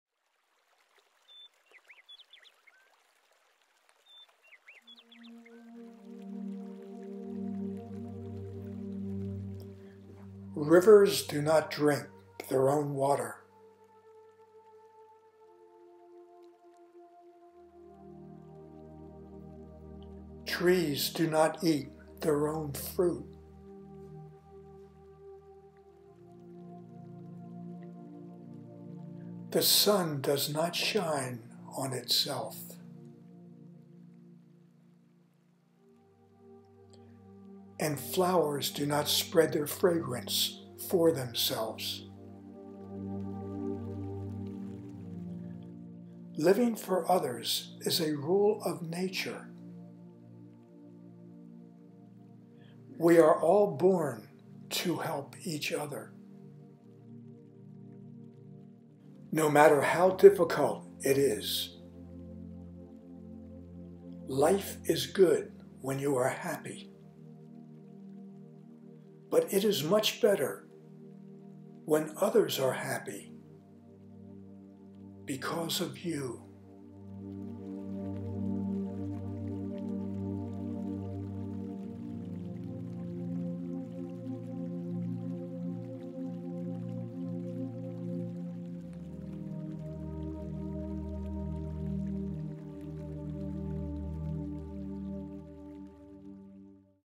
Simplicity of Life
TaiChiHealthProducts.org website shares a profoundly simple message from Pope Francis that sounds very similar to Lao Tzu's wisdom.

Video & original music - Tori Fiore Film Projects. Narration - Don Fiore